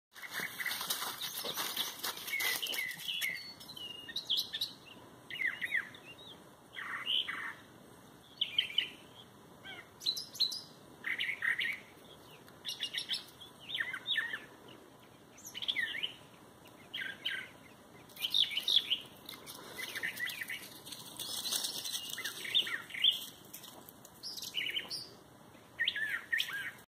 Bird sounds
I thought let u hear some bird chirping,was thinking it was our mockingbird ,but I don't think it was